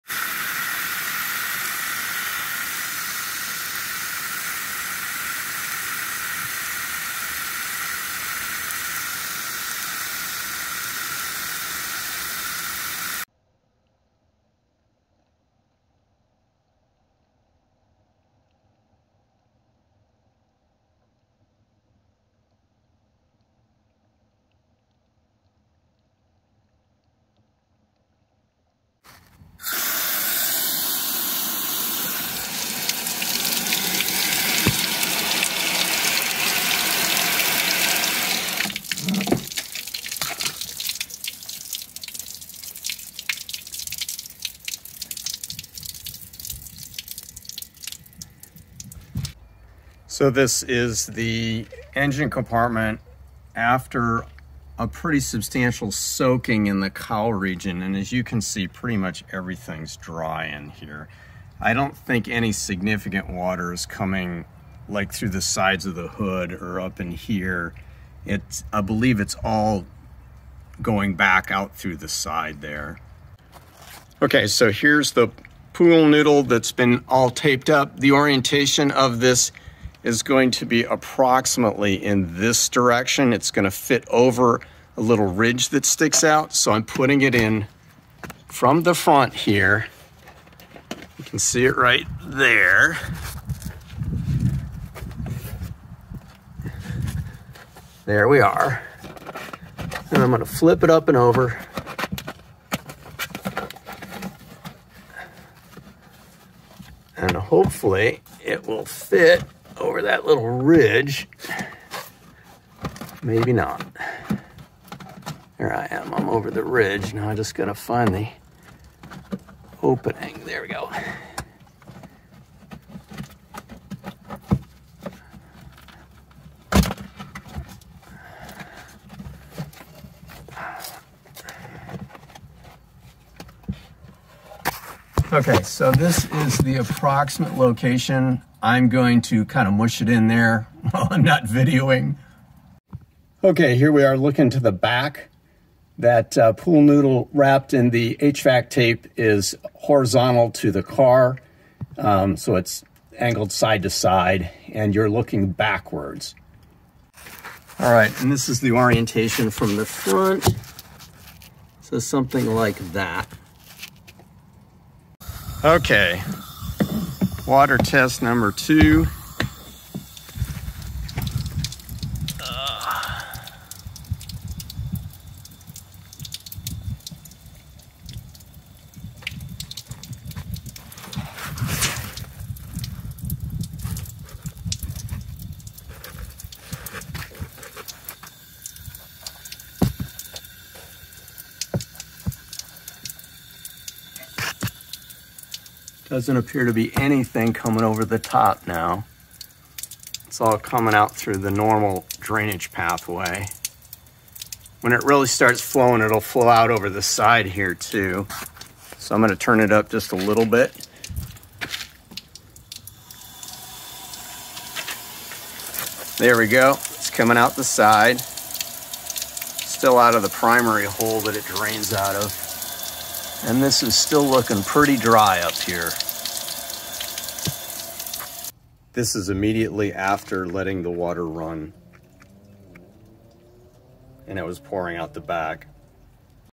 Fisker Ocean cowl leak to front electrical connector fix (frame rail blocker)
This is a companion video to the write up done on the FOA discourse site and various FOA / Fisker Facebook sites. In the video I use a industrial endoscope to video front lower right electrical connector (already waterproofed) by placing it into the underside of right front bumper while spraying vehicle in various locations from the front. Water starts to flow heavily onto right bumper area/connector while spraying the passenger side windshield/hood junction. There is a interal support beam that runs front to rear with a depression at the upper surface and the plastic cowl at windshield base is open at bottom, allowing water to flow toward the front of vehicle in this depression. This is shown at 0:45 in video.There does not appear to be significant water intrusion from the sides of hood into body.  I fashion a barrier from firm foam to fill depression and block opening under cowling (a pool noodle) and cover with Nashua metal tape (used for HVAC and other high temp applications). This barrier is attached to the front edge/shelf of the plastic cowl where it meets the support beam. The remaining video shows no water flow forward with this barrier taped in place.

Update; You can easily do this without removing the fender liner. Insert barrier through 'engine' compartment side. I also blocked off the wheel wells with an additional barrier, swapped the washer fluid sensor for new type, did a VSP block off plate and used weatherstripping around front and sides of hood. I did a reeval (video recently published) 7 months after this video showing no evidence of water flowing forward along upper frame rail and the barriers have held up well. My 'engine' compartment has stayed much cleaner than before. This appears to be an effective way to prevent water flowing from cowl to front bumper and related electrical connectors. I recommend doing both sides as water can flow onto the charging cables on left side.